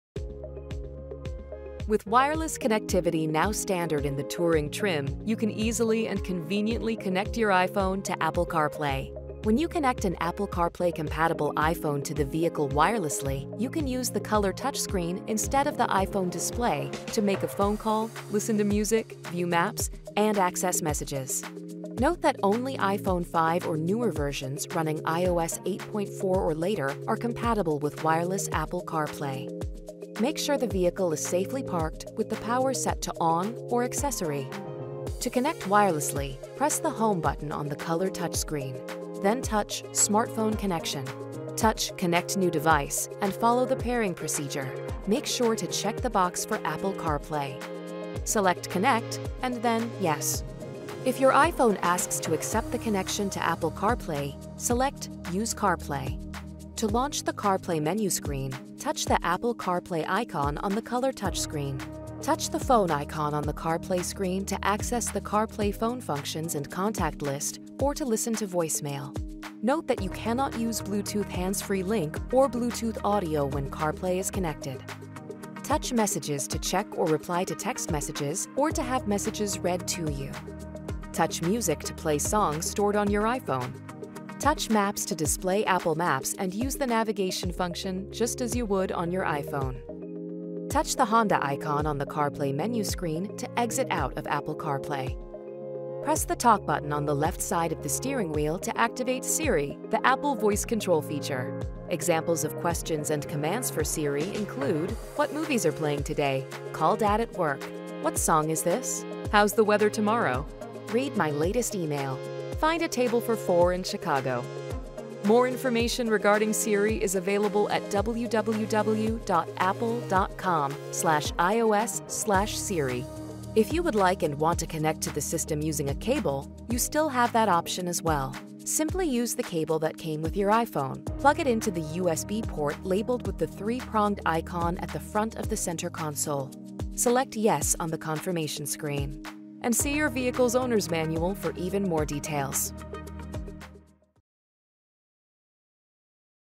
How to Connect and Use Apple CarPlay®
Apple CarPlay® compatibility helps you enjoy many of your compatible iPhone’s® functions while driving, such as making phone calls, listening to music, viewing maps and accessing messages on the color touch-screen. This video shows you how.

Applicable on: 2023 Honda HR-V EX-L, 2022 Honda Civic Sedan Touring